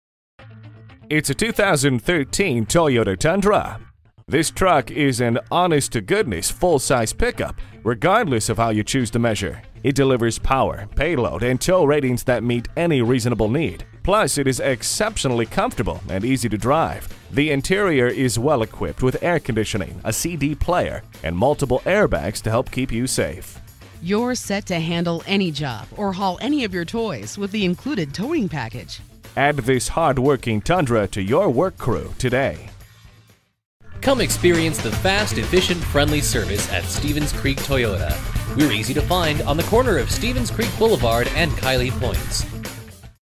2013 Toyota Tundra Regular Cab San-Jose CA Burbank, CA T131338 - SOLD
Year: 2013
Make: Toyota
Model: Tundra Regular Cab
Trim: Pickup 6 1/2 Ft
Engine: V6 4.0 Liter
Transmission: automatic
Interior: [none]
Mileage: 345
Stock #: T131338

Address:
4202 Stevens Creek Blvd